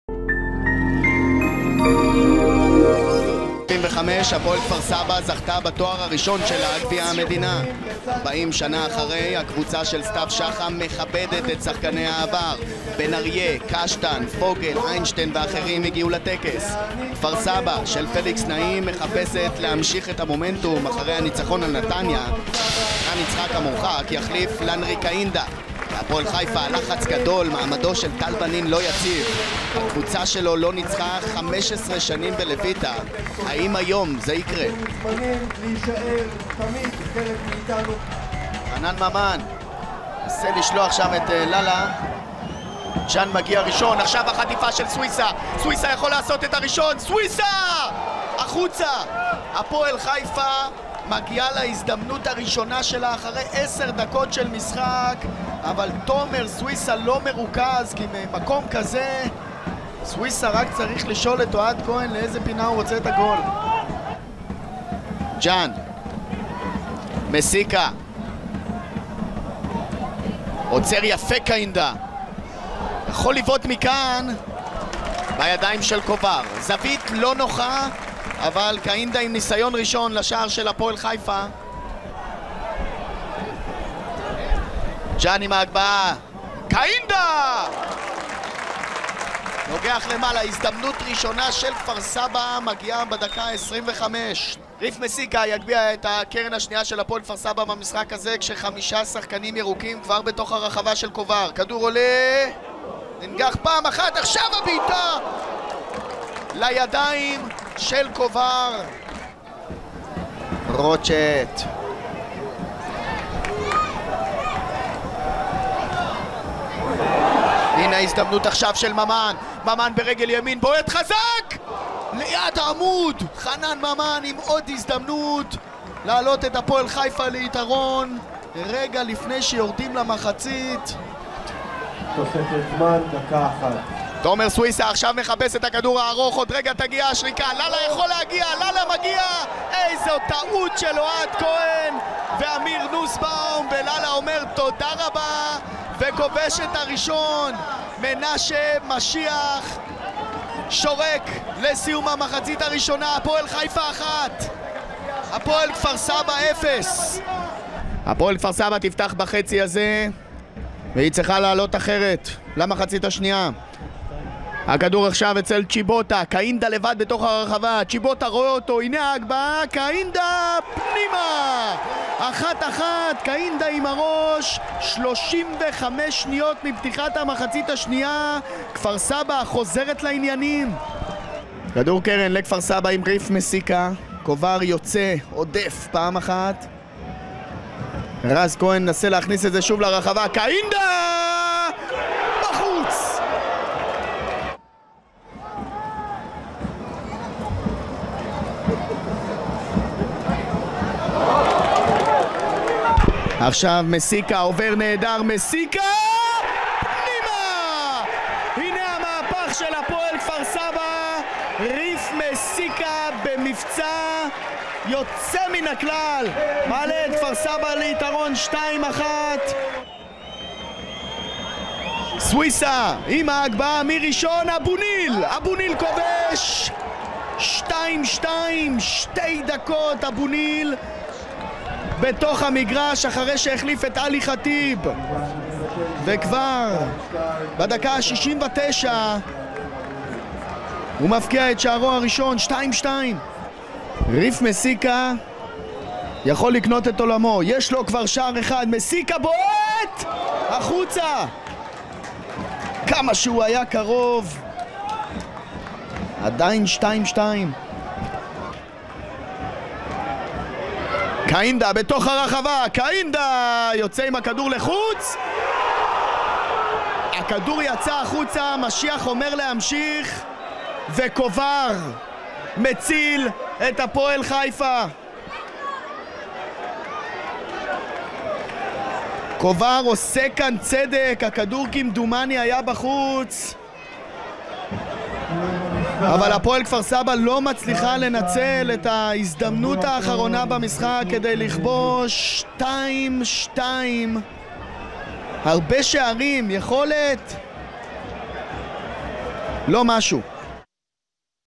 הפועל כפר סבא - הפועל חיפה 2:2, תקציר המשחק. 24.10.15
משחק מצוין בלויטה הסתיים בחלוקת נקודות. לאלה נתן לחיפאים יתרון, קהינדה ומסיקה הפכו אך אבו ניל כפה שוויון. פליקס נעים: "לא מרוצה מהנקודה"

Hapoel Kfar Saba and Hapoel Haifa finish their match with the score of 2:2

הרכבים:
הפועל כפר סבא: אוהד כהן, לירן ג'אן, דינו שקבורץ', עמיר נוסבאום, סמי אליקים, אלירן סיימון, רז כהן, ריף מסיקה (עומר פדידה, 82'), הוגו לופס (יאניס פאפאדופולוס, 75'), מאוויס צ'יבוטה (עידן דוד, 90') ולנרי קהינדה.

הפועל חיפה: פרמיסל קובאר, אופק פישלר, טאדאס קיאנסקאס, אושרי רואש, מיקי יאזו, יוסי דורה, אדריאן רוצ'ט, עלי חטיב (אמיר אבו ניל , 67'), תומר סוויסה, חנן ממן (דור מלול, 90') ומהראן לאלה (זרקו קוראץ', 86').